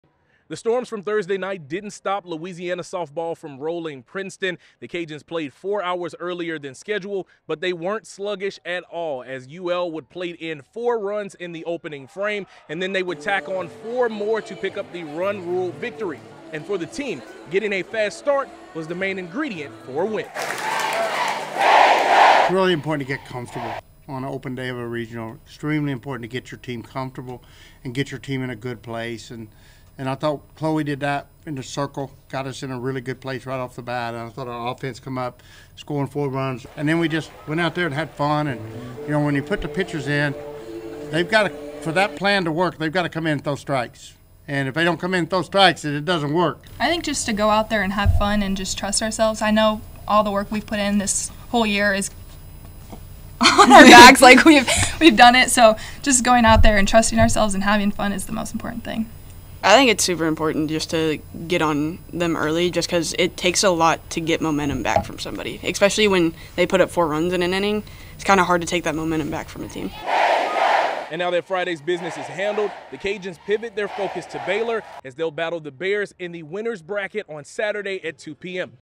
See how the Ragin' Cajuns cruised by Princeton
A trio of batters produced the bulk of the runs and a trio of pitchers held down Princeton as the No. 13 national seed Louisiana Ragin' Cajuns captured an 8-0 win in the NCAA Lafayette Regional opener